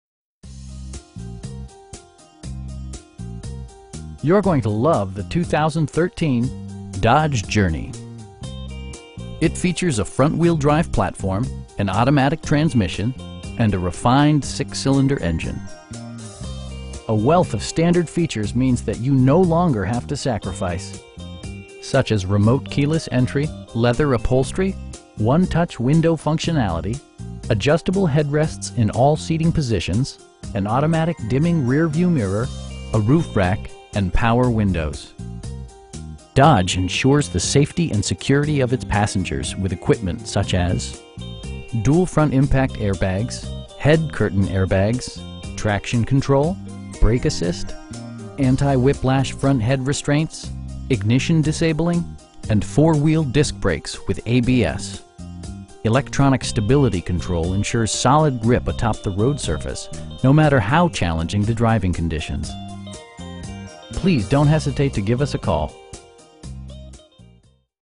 2013 Dodge Journey Crew in Highland, MI 48356
Szott M-59 Dodge
2565 East Highland Road

You're going to love the 2013 Dodge Journey. It features a front-wheel-drive platform, an automatic transmission, and a refined 6 cylinder engine. A wealth of standard features means that you no longer have to sacrifice. Such as remote keyless entry, leather upholstery, 1-touch window functionality, adjustable headrests in all seating position an automatic dimming rear-view mirror, telescoping steering wheel, a roof rack, and power windows. Dodge ensures the safety and security of its passengers with equipment such as: dual front impact airbags, head curtain airbags, traction control, brake assist, anti-whiplash front head restraints, ignition disabling, and 4 wheel disc brakes with ABS. Electronic stability control ensures solid grip atop the road surface, no matter how challenging the driving conditions. We have the vehicle you've been searching for at a price you can afford. Please don't hesitate to give us a call.